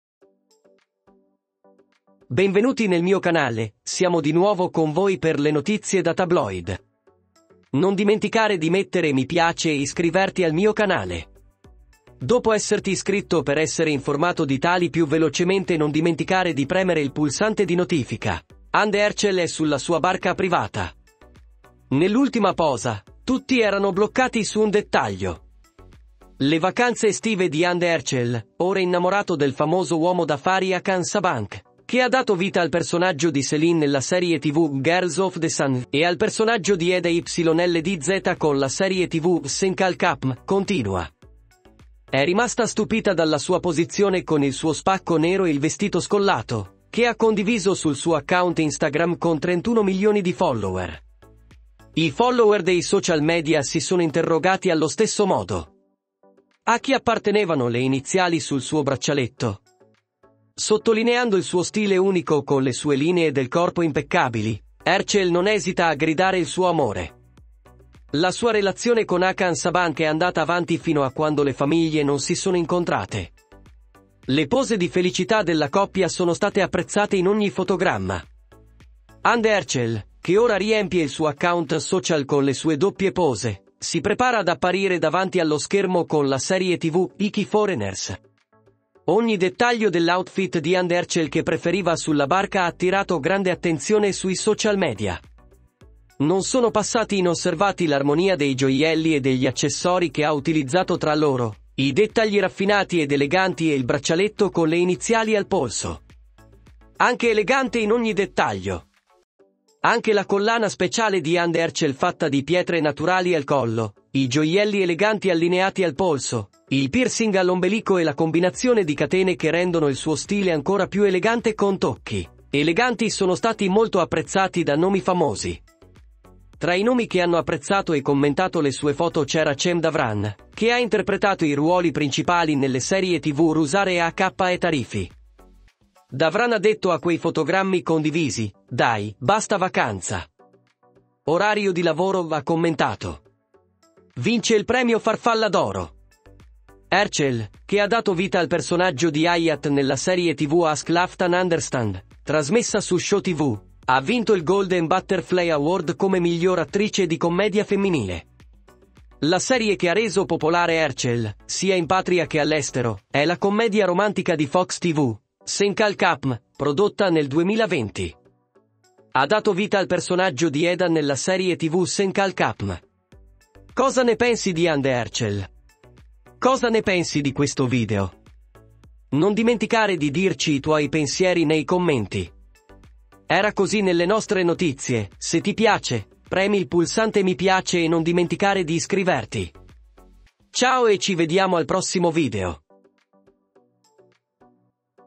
Tutti erano rimasti bloccati su un dettaglio della posa di Hande Erçel sulla sua barca privata!
In questa notizia, "Tutti erano rimasti bloccati su un dettaglio della posa di Hande Erçel sulla sua barca privata!" Abbiamo spiegato l'argomento. Grazie.

Parti del video;
00:00 - Ingresso
00:13 - Hande Erçel
03:06 - Fine